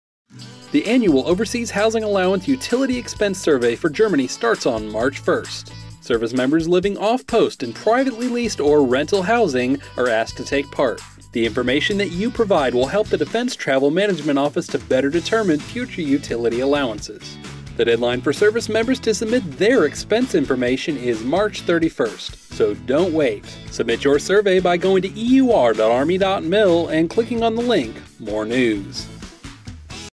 OHA Utility Expense Survey for Germany: OLD Version
U.S. Army Europe is asking service members in Germany, Moldova, the Netherlands and Russia who are residing off-post in privately leased or rental housing to take part in the annual housing survey, from March 1-31, 2016.